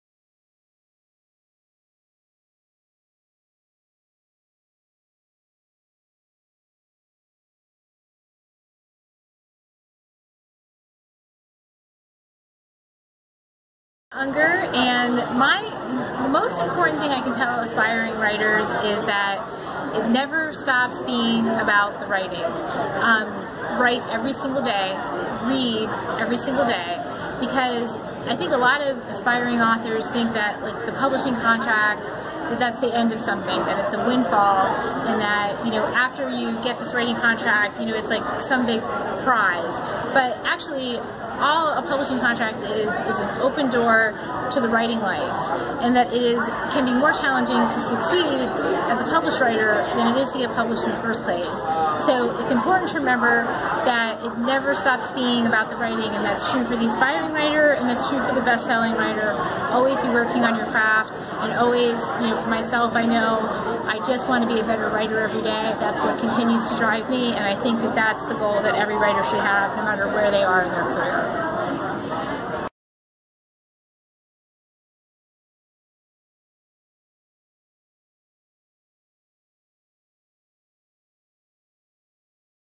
SinC Up with Lisa Unger
Sisters in Crime, Lisa Unger, author, writing tip is to write every day